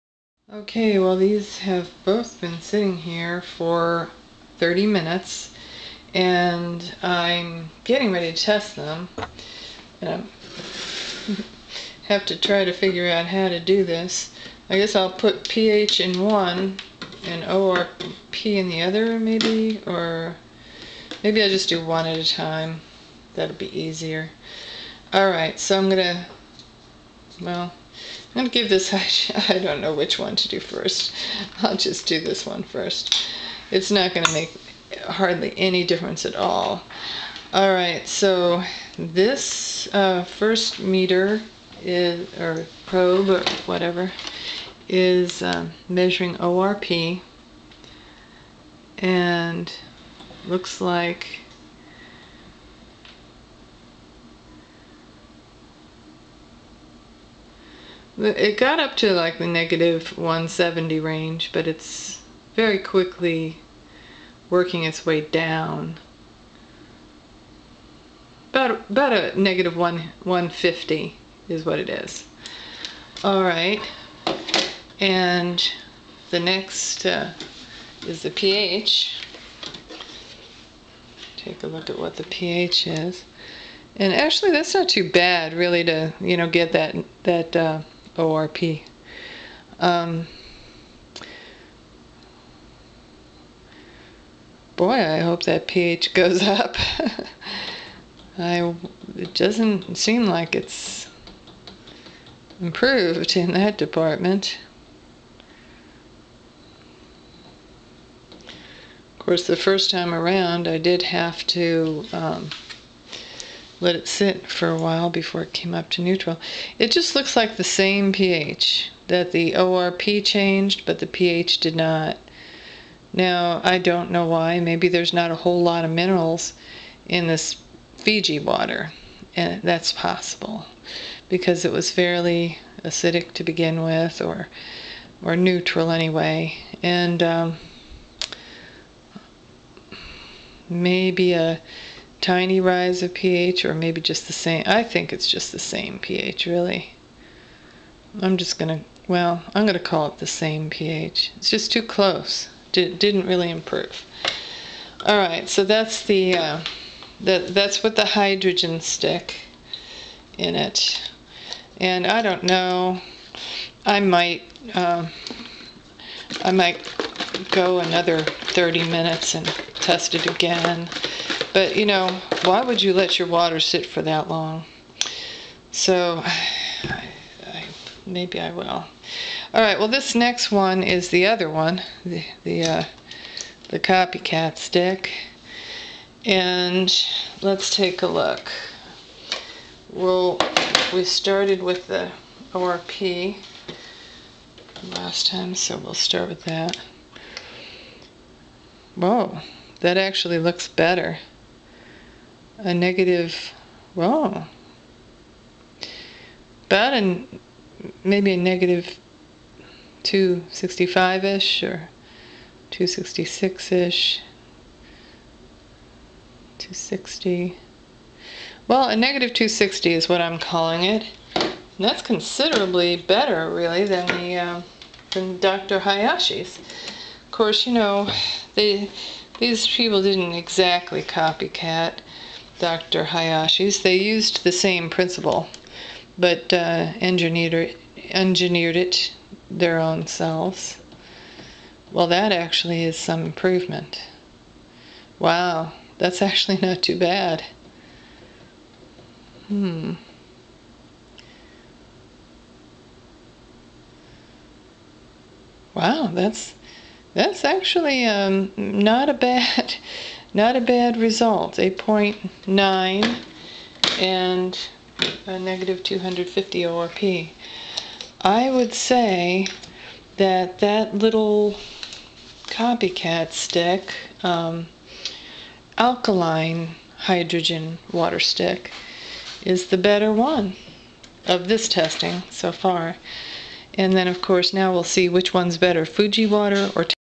comparing-two-brands-hydrogen-stick2.mov
, is testing two brands of the Hydrogen Stick. One brand is the one which was developed by Dr. Hayashi, called the Hydrogen Water Stick. The other brand is a copy-cat Hydrogen stick which has an outer shell of stainless steel and contains beads of magnesium [vs the solid magnesium in Dr. Hayashi's stick. There are several videos in a series. Cathie tested the Fiji water first for pH and ORP. Then she soaked the two H-sticks in Fiji water for 30 min. and re-tested. Then she let the bottles sit for another hour and re-tested. Then she tested the pH and ORP of her St. Louis tap water. Finally, she tested the two Hydrogen sticks after soaking in tap water for 30 minutes. Cathie does lots of testing on alkaline water, ionized water and tests various water ionizers. You can read the full article on this series of tests in her blog, ...look for the article to appear in the end of July, 2010.